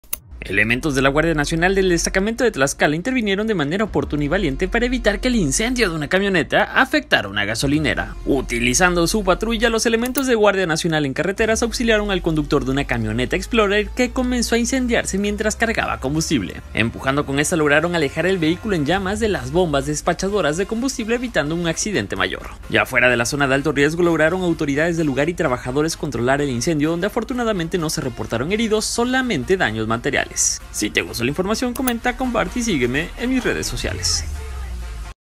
🔥Guardia Nacional ayuda a sofocar incendio de camioneta en gasolinera de Tlaxcala, Mexico.
Elementos de la Guardia Nacional, destacamento Tlaxcala, intervinieron de manera oportuna y valiente para evitar que el incendio de una camioneta afectara una gasolinera.